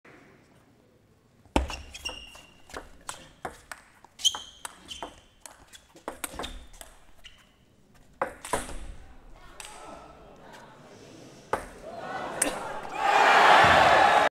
Neue Taktik: Punkt machen ❌ Gegner abschießen ✅
Bastian Steger vergibt diesen Schmetterball am vergangenen Spieltag und schießt stattdessen seinen Gegner Ruwen Filus ab